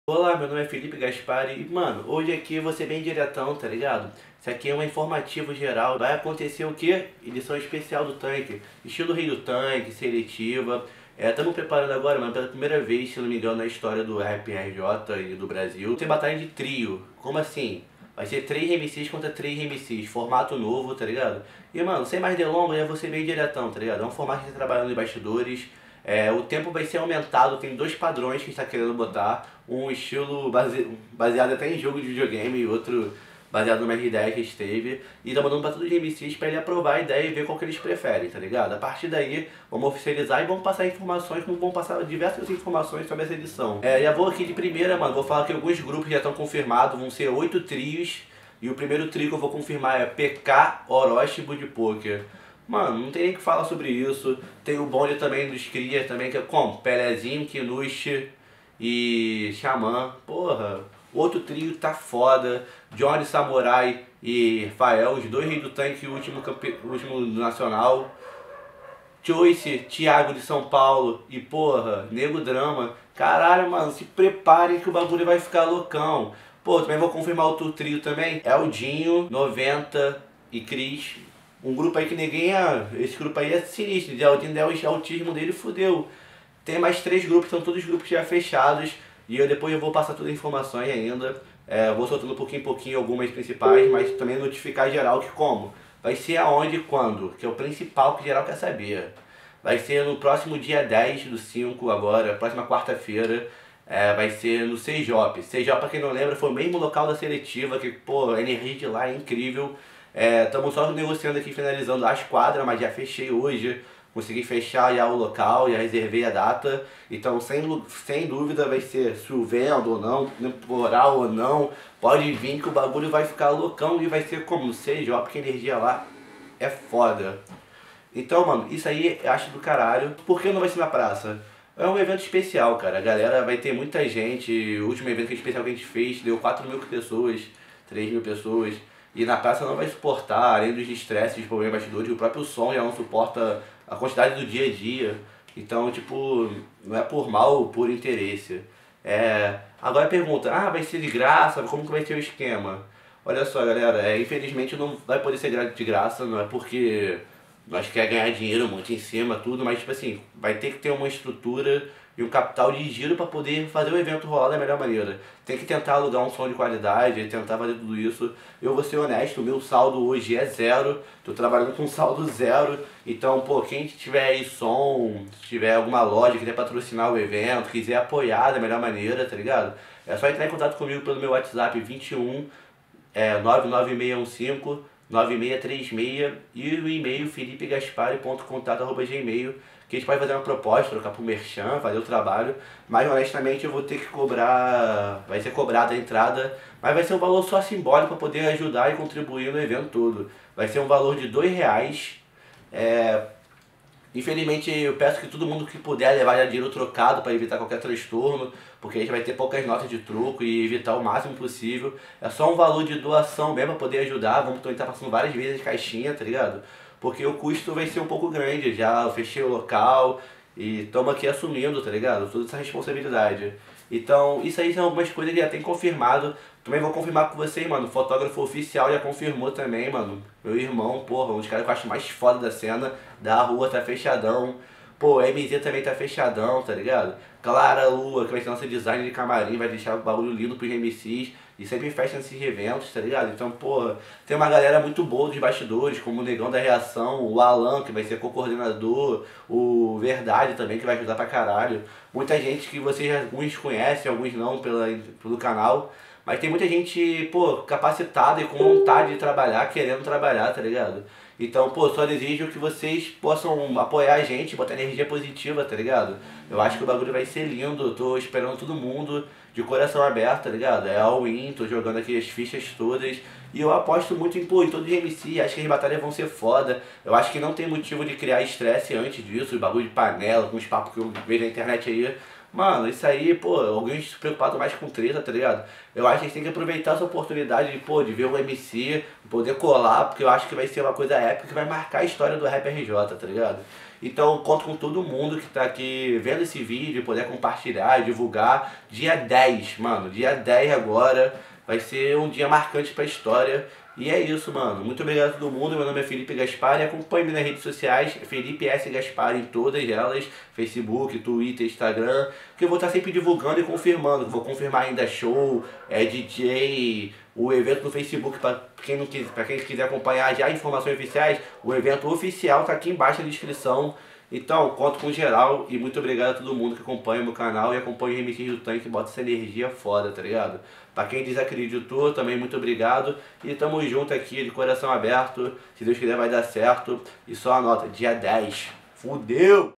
10/05 - Batalha de Trio [ Felipe Gaspary ] [ INFORMAÇÕES ]
Informações sobre a batalha de trio que acontecerá no dia 10/05 edição especial da BDT